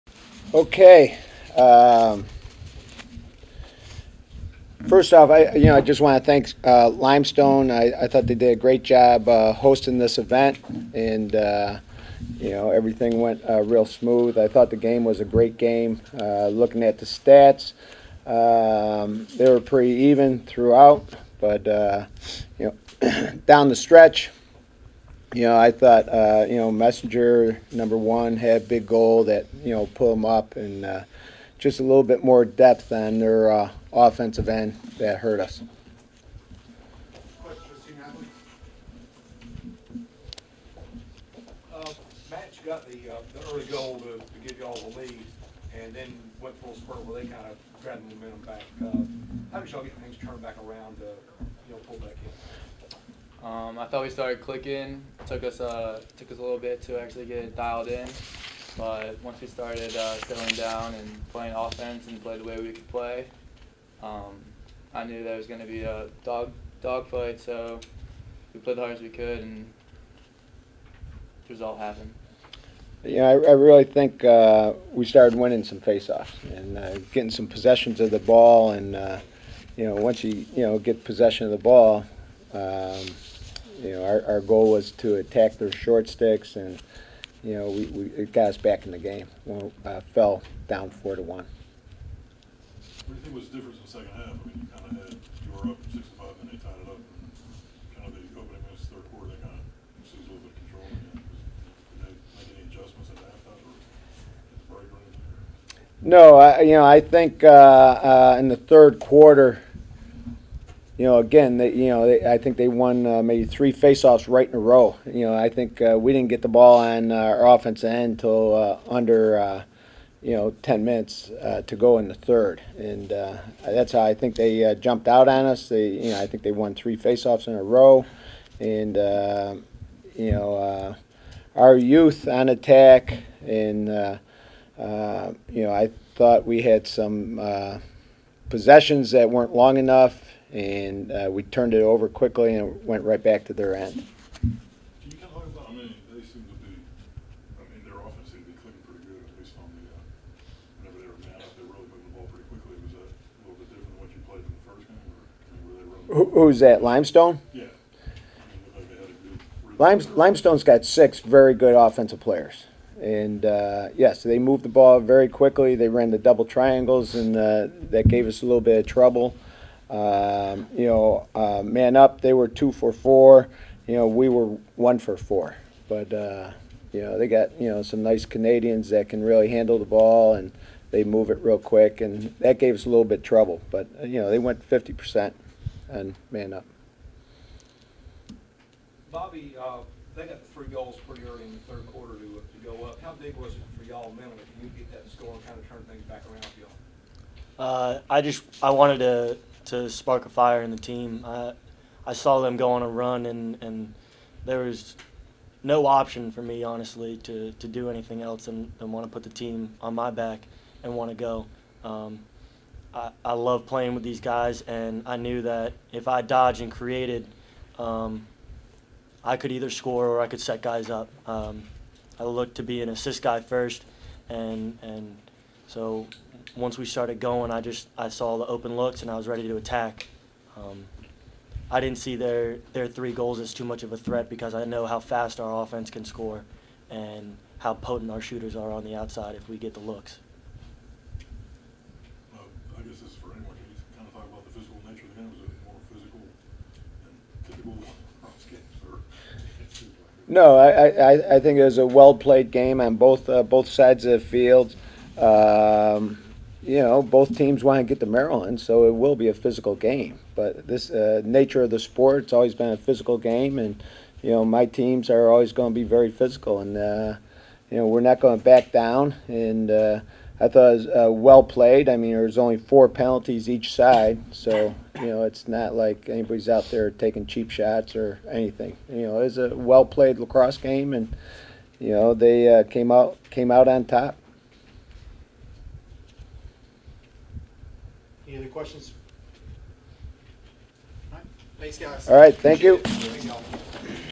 No. 3 Tampa Men's Lacrosse NCAA Semifinals Postgame Press Conference
Head Coach Rory Whipple and student-athletets Matt Bilak and Bobby Calhoun recount their NCAA Division II semifinals loss to No. 2 Limestone College, 14-10.